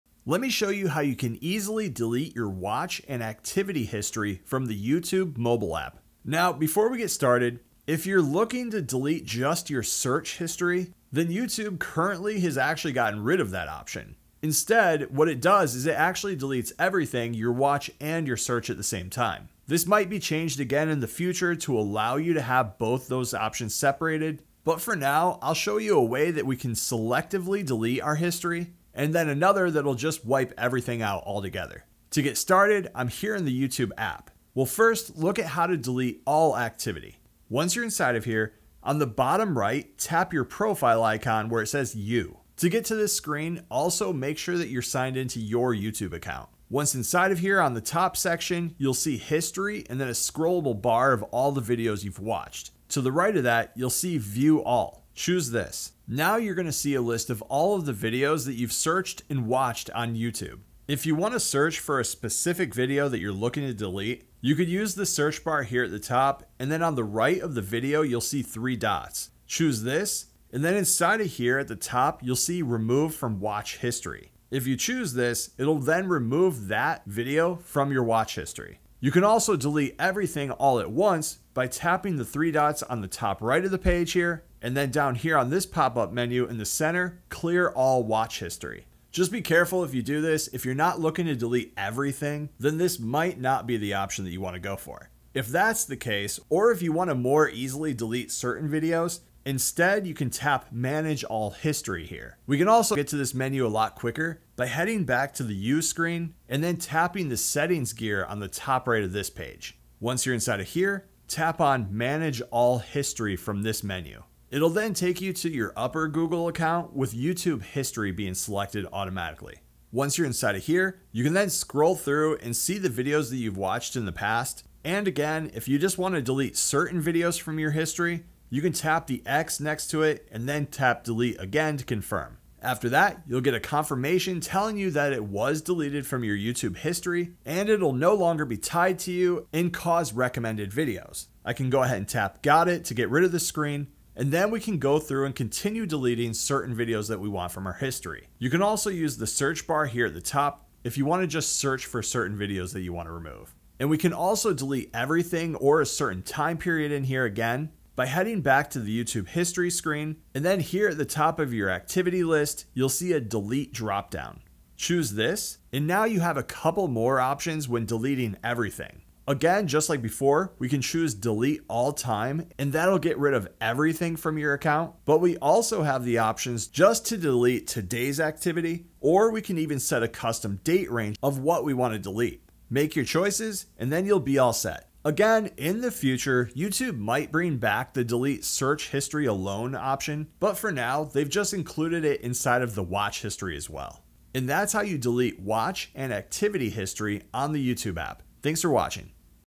How To Delete Search History on YouTube App
This is how you can easily delete all or specific videos from your YouTube app's history. We'll look at a few options we have to do this with this video. Here's how to manage your activity:

Deleting Specific Videos:

Open the YouTube app: Make sure you're signed in to your account.
Tap Profile Picture: In the bottom right corner, tap your profile picture.
Go to History: Tap "History" and then "View all" next to your watch history.
Find Video: Use the search bar or scroll through the list to find the video you want to remove.
Tap Three Dots: Locate the three dots next to the video title.
Select "Remove from watch history": This will remove the chosen video from your watch history.
Deleting All Watch History:

Method 1: From History Screen

Follow steps 1-3 above (Open YouTube app - Profile Picture - History).
Tap Three Dots: Tap the three dots in the top right corner of the History screen.
Choose "Clear watch history": This will delete your entire watch history.
Method 2: From Settings

Open the YouTube app and tap Settings gear: Locate the settings gear icon in the top right corner.
Select "Manage all history": Choose "Manage all history" from the Settings menu.
Switch to YouTube History (if needed): YouTube history should be automatically selected.
Delete Specific Videos (optional): Tap the "X" next to videos you want to remove and confirm with "Delete."
Delete All Watch History: Tap the three dots in the top right corner and choose "Clear watch history."
Deleting by Time Range:

Tap the "Delete" dropdown: Locate the "Delete" dropdown menu at the top of your activity list.
Choose Time Range: Select "Delete today's activity" or set a custom date range to delete videos within that period.